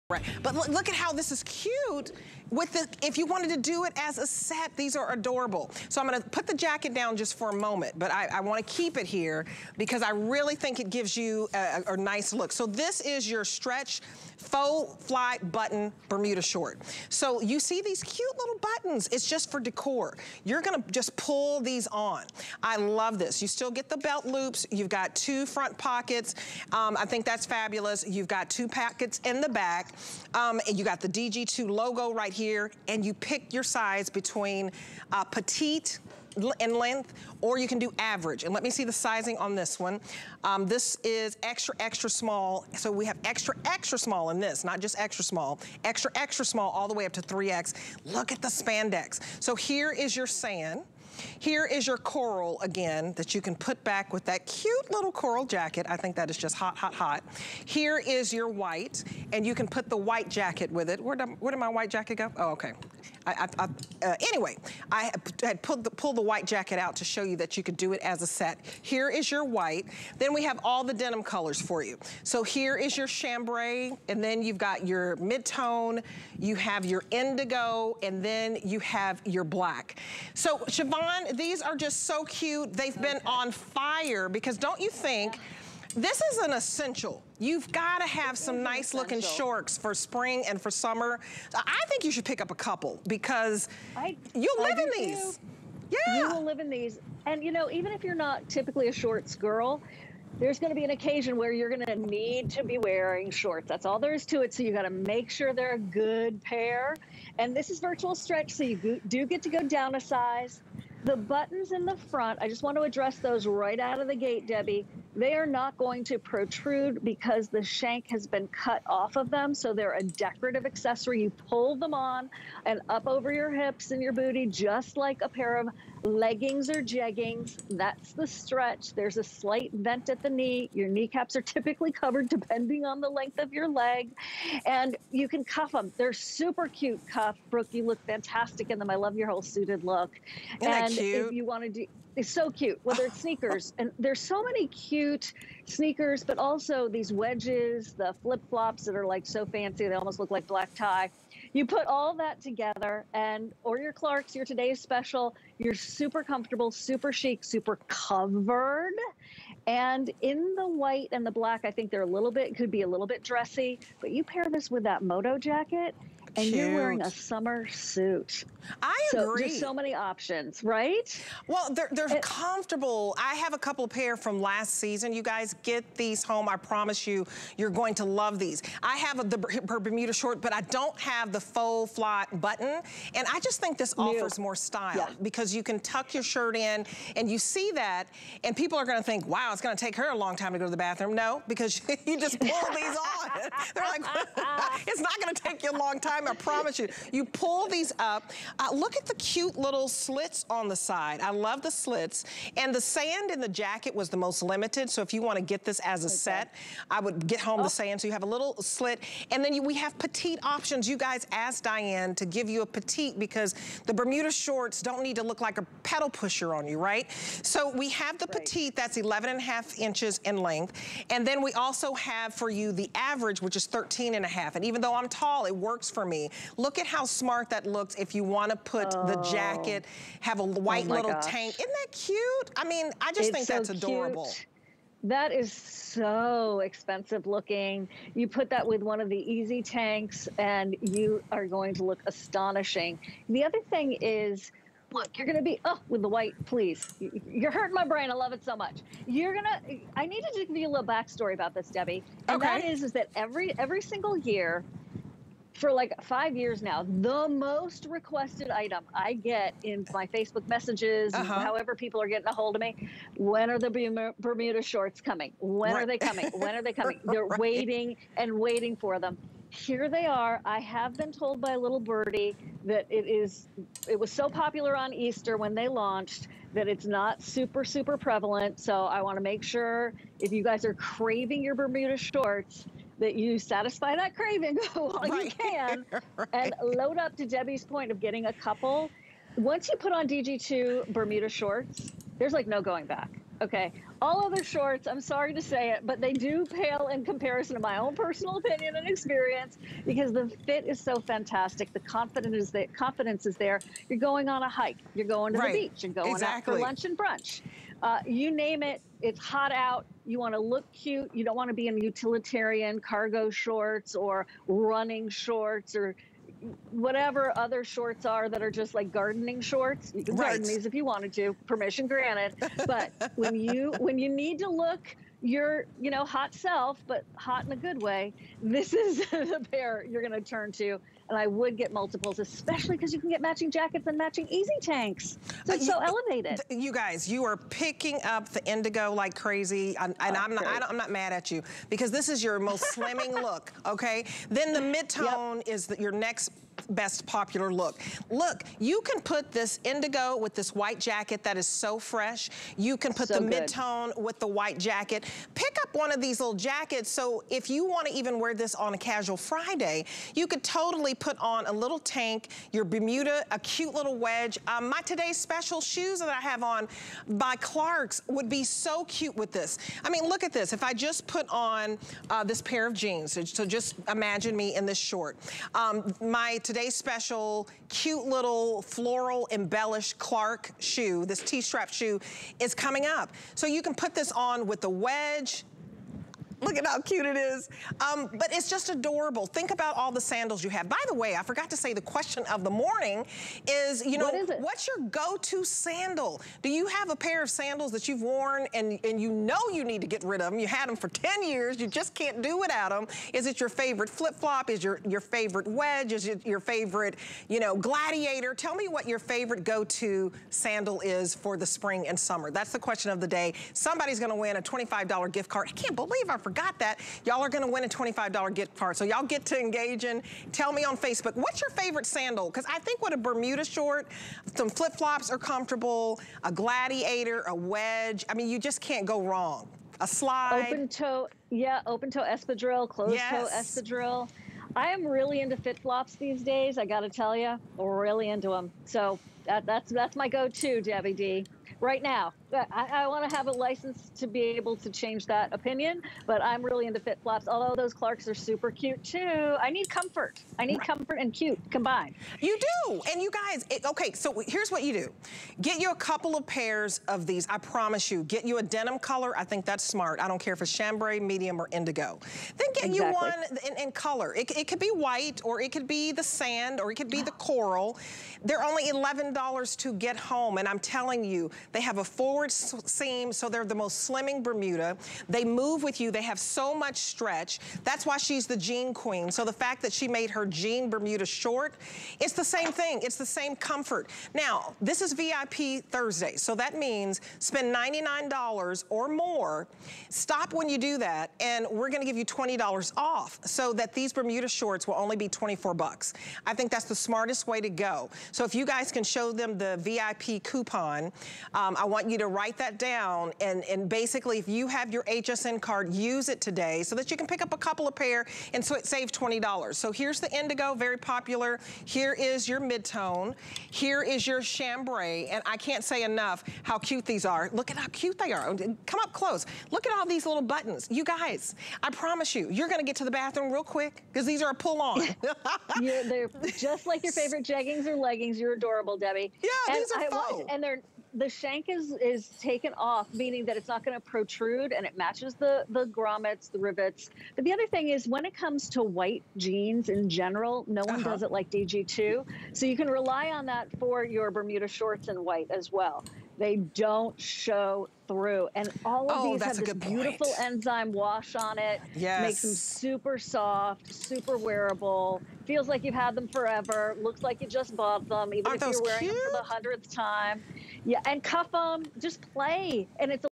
DG2 by Diane Gilman Faux Button Fly Bermuda Short Basic
DG2 by Diane Gilman Faux Button Fly Bermuda Short
Build an ohsocomfortable summer wardrobe around pieces like these virtual stretch denim Bermuda shorts. A faux button fly and functional pockets bring classic jean detailing to the effortless style.